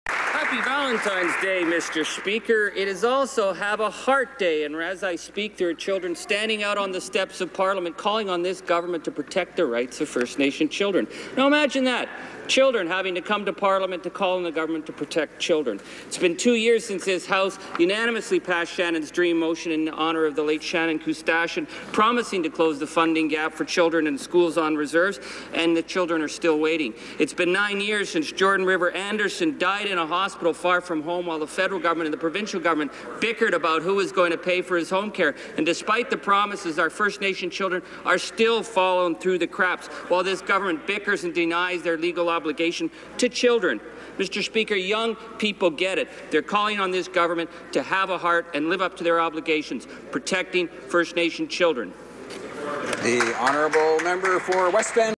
Charlie Angus on have a heart day
Timmins James Bay MP Charlie Angus calls on the conservatives to treat first nations children fairly, just the same as others